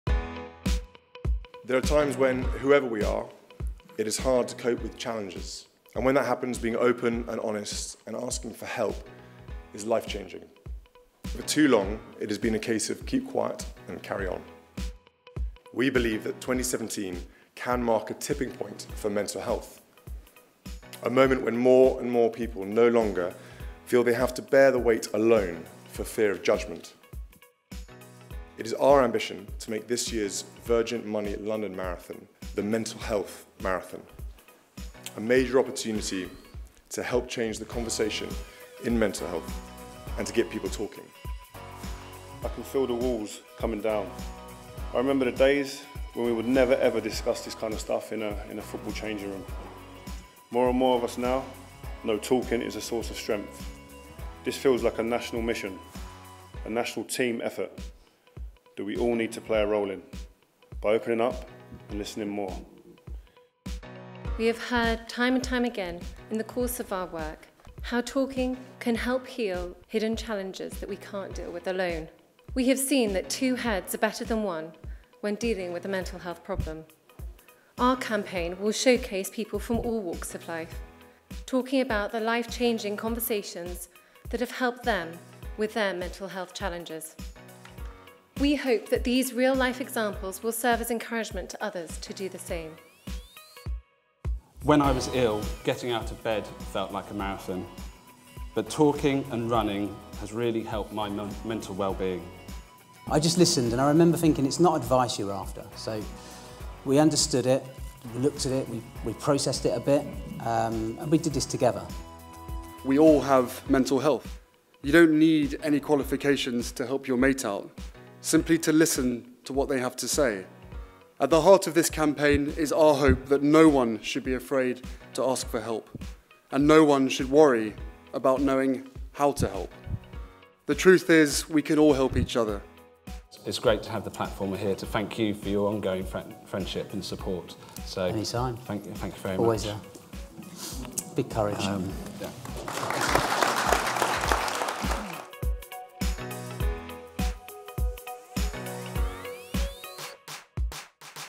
Heads Together | 2017 Ambition
At today’s briefing for leaders from across UK society, Their Royal Highnesses said that 2017 marks a ‘tipping point’ for mental health in the UK and outlined how Heads Together aims to make this the year when millions of people talk about their mental health without fear of judgement.